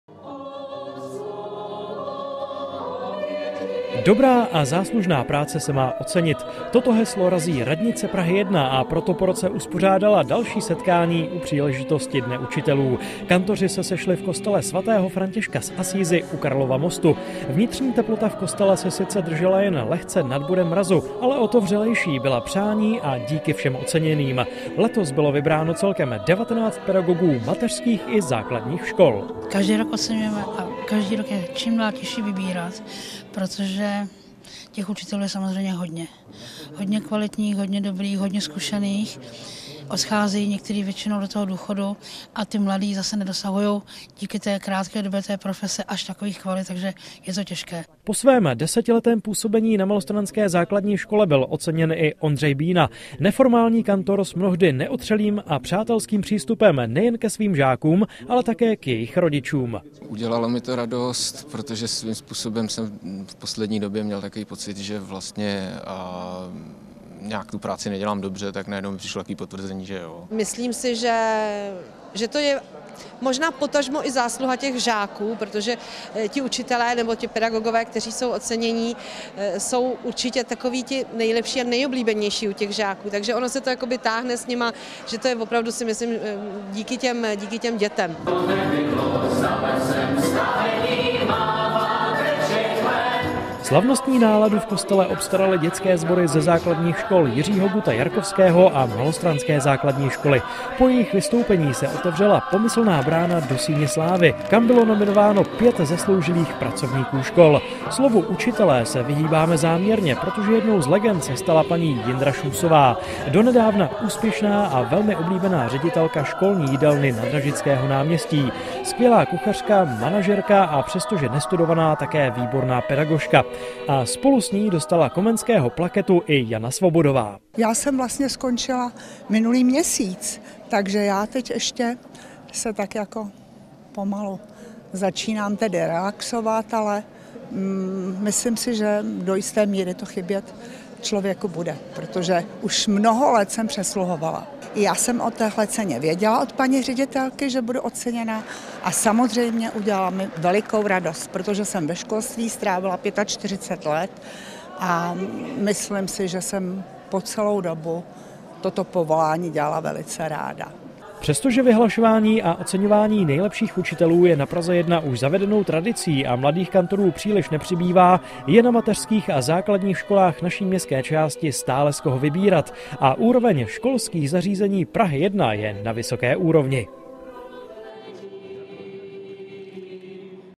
Den učitelů na Praze 1
Radnice první městské části u příležitosti Dne učitelů opět rozdávala ocenění nejlepším pedagogům. Letos jich z mateřských a základních škol bylo vybráno 19. Do Síně slávy postoupilo dalších 5 kantorů i nepedagogických pracovníků škol.